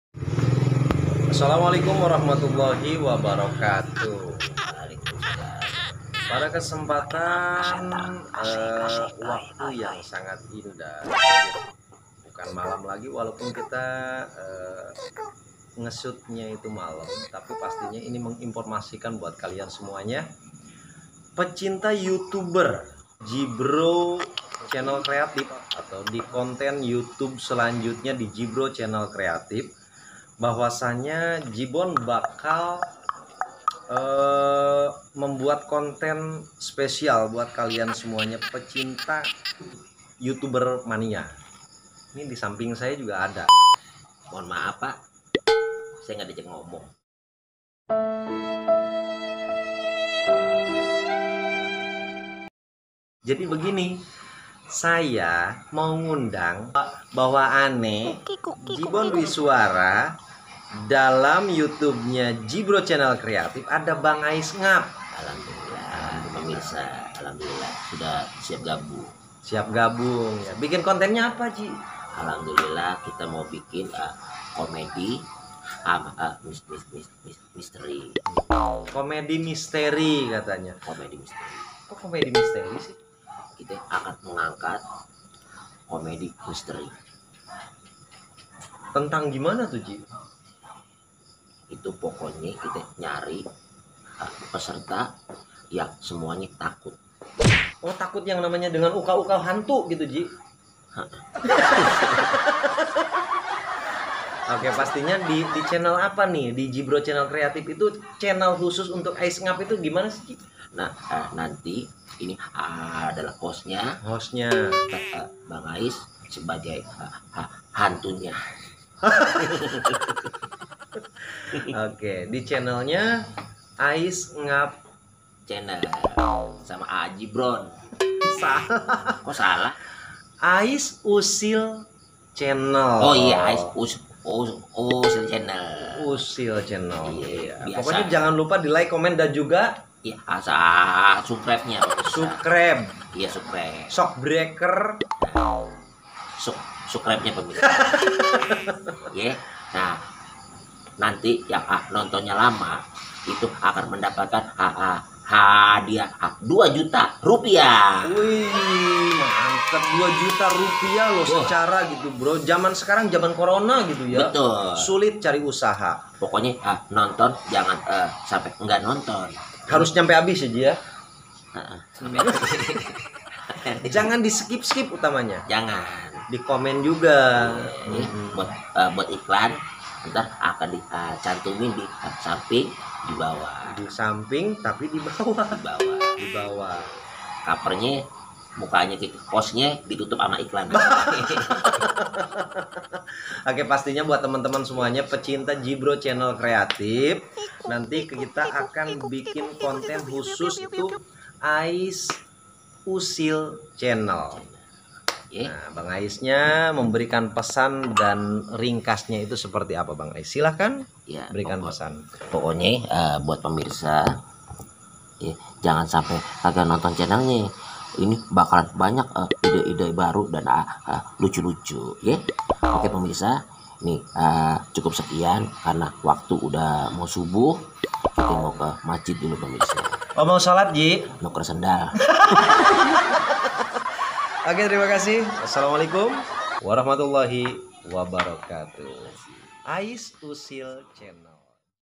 Bang AIS NGAP | Mau Bikin Kejutan Di Perum RELIEF GREENVILLE Cileungsi
Salam Seniman Entertainment...
Alhamdulillah Bang AIS NGAP Akhirnya Siap Bikin Rencana Kerja Bareng ZHIBRO Channel Creative, Dan Siap Buat KEJUTAN Warga Perum Relief Greenville Cileungsi

Tunggu Waktu Tepat Aje Ye...?!
Kembaran Ajis Gagap